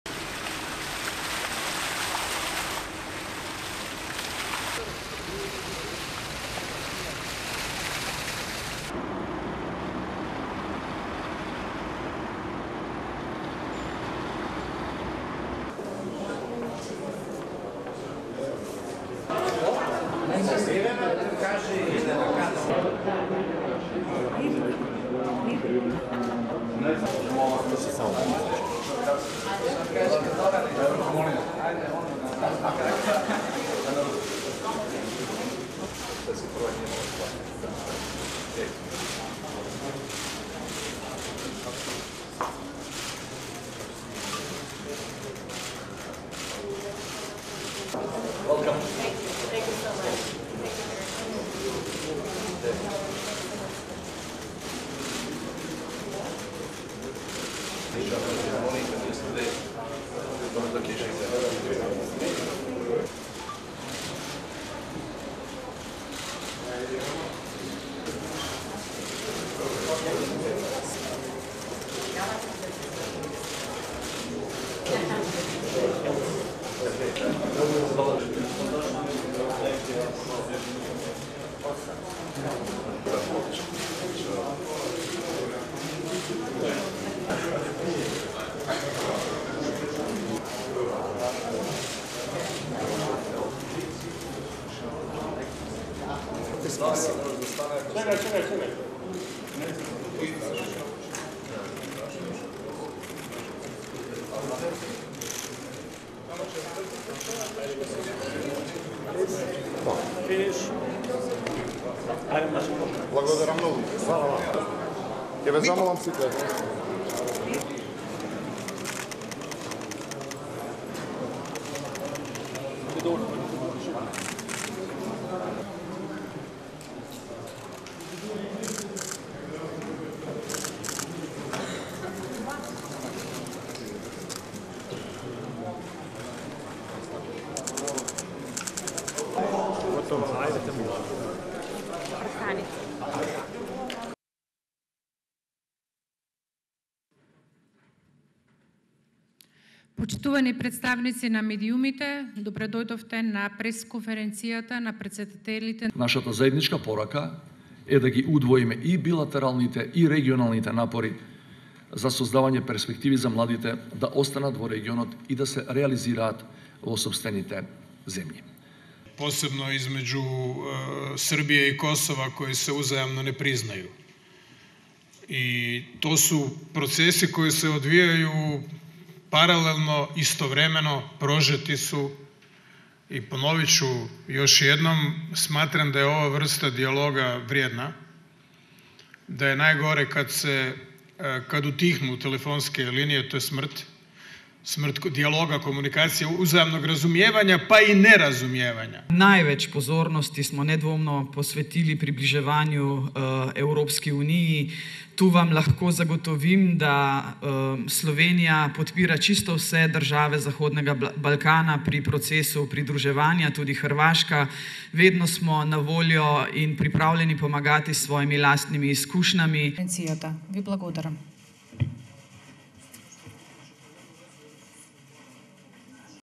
Regional leaders attend meeting of Brdo Brijuni Process in North Macedonia
RESTRICTION SUMMARY:

ASSOCIATED PRESS
Skopje, North Macedonia - 11 September 2023
1. Wide of venue, Hilton Hotel
2. Various of police officers outside hotel
3. Sign reading “Brdo – Brijuni Process”
4. Mid of media
5. Croatia’s President Zoran Milanovic, left and Slovenia’s President Natasa Pirc Musar, right
6. North Macedonia’s President Stevo Pendarovski, right, Croatia’s President Zoran Milanovic, left and Slovenia’s President Natasa Pirc Musar, centre, shake hands
7. Pendarovski, welcomes Serbia’s President Aleksandar Vucic, left
8. Vucic poses with presidents of Croatia, Slovenia and North Macedonia
9. North Macedonia’s President Stevo Pendarovski, right, welcomes Kosovo’s President Vjosa Osmani Sadriu, left
10. Kosovo’s President Vjosa Osmani Sadriu poses with Presidents of Croatia, Slovenia and North Macedonia
11. North Macedonia’s President Pendarovski welcomes Albania’s President Bajram Begaj, left
12. Albania’s President Bajram Begaj poses with Presidents of Croatia, Slovenia and North Macedonia
13. North Macedonia’s President Pendarovski welcomes Montenegro’s President Jakov Milatovic, left
14. Montenegro’s President Jakov Milatovic poses with Presidents of Croatia, Slovenia and North Macedonia
15. North Macedonia’s President Pendarovski welcomes the member of the Presidency of Bosnia and Herzegovina Zeljko Komsic, left
16. The member of the Presidency of Bosnia and Herzegovina Zeljko Komsic poses with Presidents of Croatia, Slovenia and North Macedonia
17. North Macedonia’s President Pendarovski welcomes the member of the Presidency of Bosnia and Herzegovina Denis Becirovic, left
18. The member of the Presidency of Bosnia and Herzegovina Denis Becirovic poses with Presidents of Croatia, Slovenia and North Macedonia
19. North Macedonia’s President Pendarovski welcomes the member of the Presidency of Bosnia and Herzegovina Zeljka Cvijanovic, left
20. The member of the Presidency of Bosnia and Herzegovina Zeljka Cvijanovic poses with Presidents of Croatia, Slovenia and North Macedonia
21. Family photo of the participants
22. Panning from left during the family photo
23. Serbia’s President Aleksandar Vucic and Croatia’s President Zoran Milanovic walking
24. Wide of the round table
25. Panning on the participants on the meeting
26. Wide of the round table
27. Joint news conference of presidents of Croatia, North Macedonia and Slovenia
28. SOUNDBITE (Macedonian) Stevo Pendarovski, North Macedonia’s President:
“Our joint message is to double both bilateral and regional efforts to create perspective for the young people to remain in the region and to realise themselves in their own countries”
29. SOUNDBITE (Croatian) Zoran Milanovic, President of Croatia:
“Especially (the relationship in the region) between Serbia and Kosovo, which does not recognise each other and there are other processes going on at the same time. I’ll repeat once again that this kind of dialogue is valuable. It is the worst when the phone lines stop, it is death for the dialogue in understanding and in the misunderstanding.”
30. SOUNDBITE (Slovenian) Natasa Pirc Musar, President of Slovenia:
“We undoubtedly paid the most attention to the accession to the European Union. I can assure you here that Slovenia is supporting all the countries of the Western Balkan in the process of the accession, also Croatia, we are always available and ready to help with our experiences.”
31. End of the news conference
STORYLINE:

Dialogue between Serbia and Kosovo was encouraged by Croatian President Zoran Milanovic.